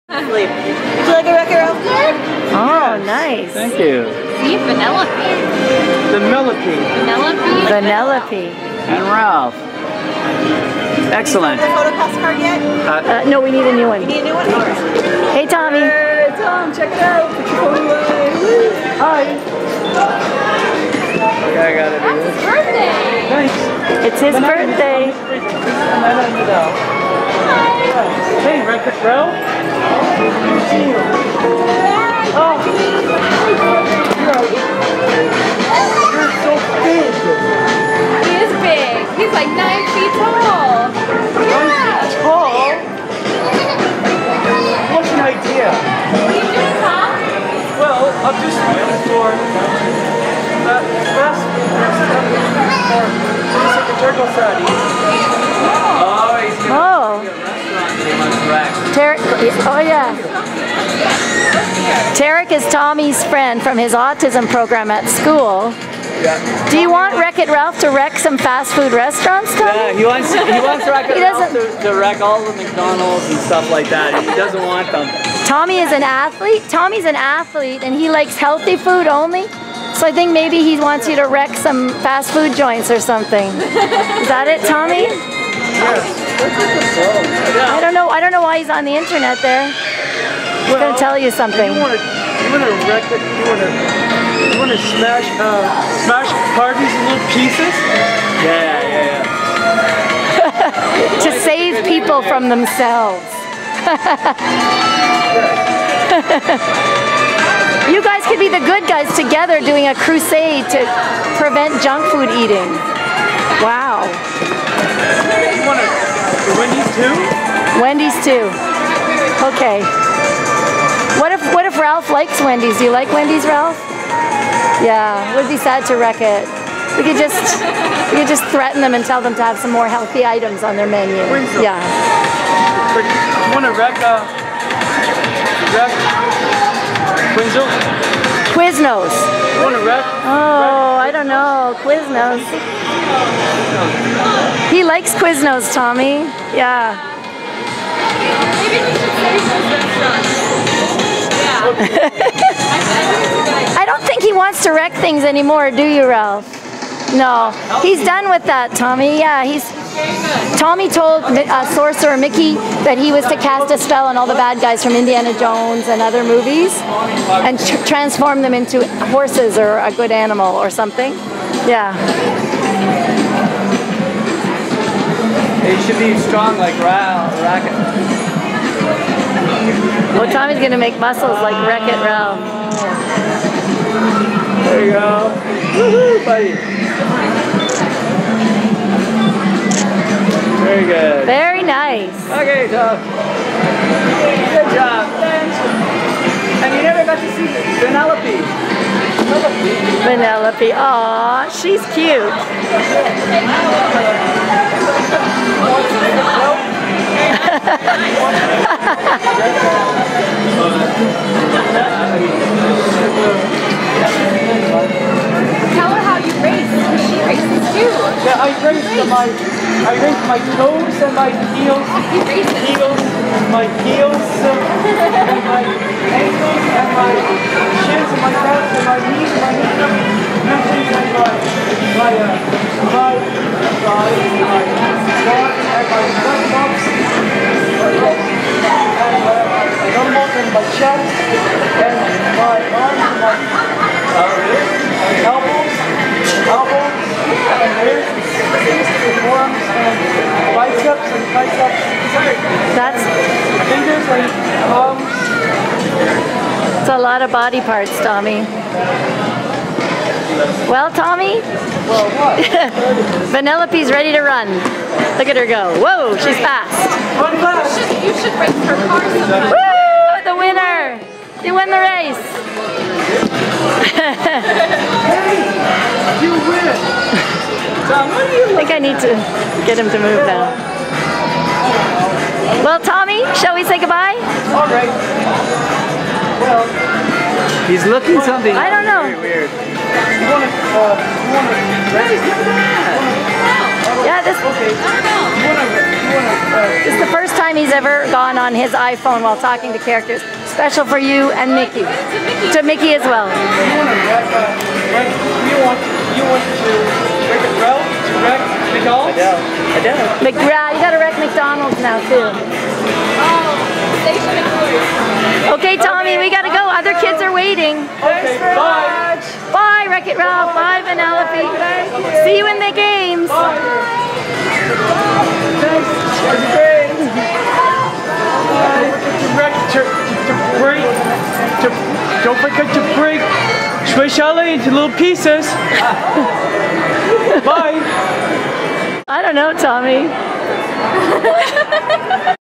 Tommy meets Wreck-It Ralph and Vanellope
Tommy asks Wreck-It Ralph to help him get rid of junk food (Tommy loves healthy food!) and talks with Vanellope about racing!!!